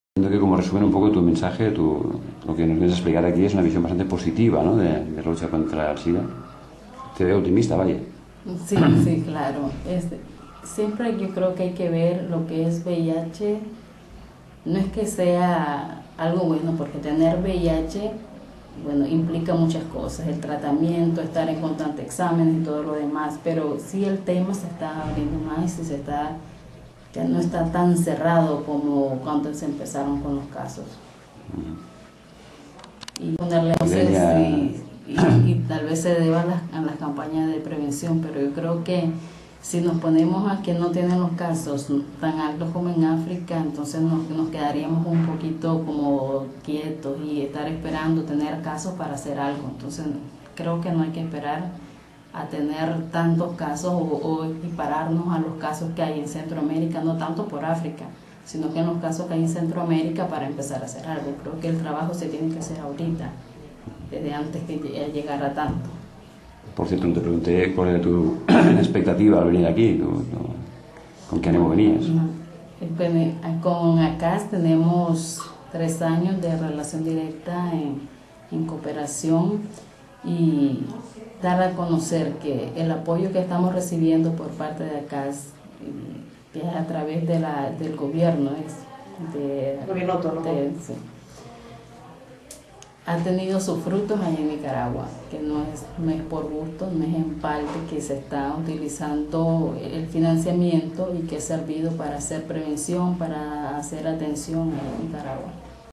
Yanina Miranda (ANICP-Vida) i la lluita contra la Sida a Nicaragua (1)
Yanina Miranda, de l'associació ANICP-Vida, ha estat convidada a Barcelona per l'Associació Ciutadana Anti-Sida de Catalunya, per parlar de la situació del combat contra aquesta malaltia a Nicaragua.